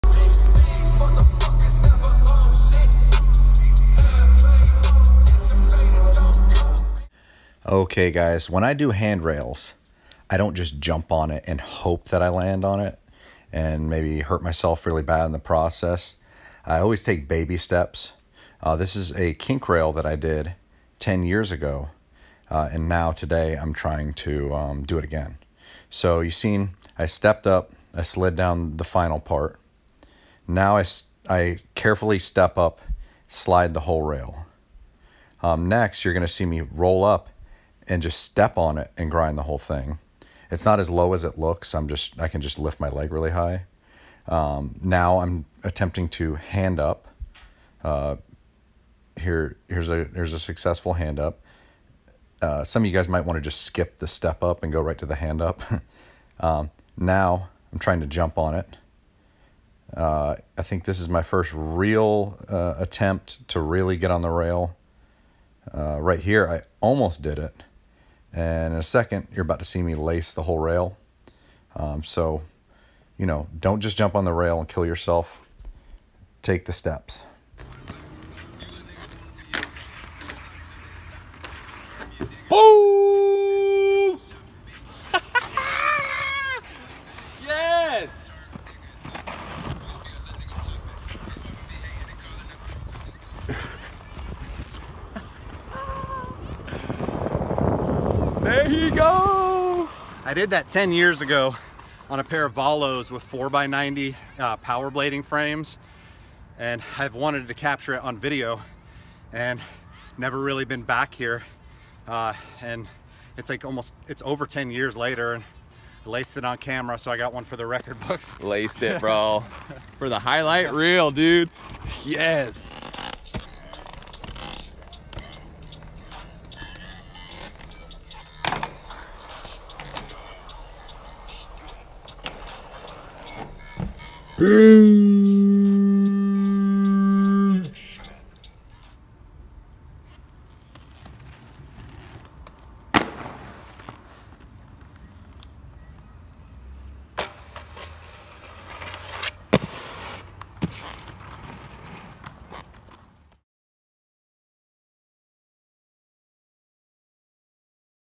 How to grind a handrail without injuries!!
Most people just jump on the rail and hope for the best... in this video I detail my personal process on how to grind a handrail without being injured!

Blank SK Beta skate by Rollerblade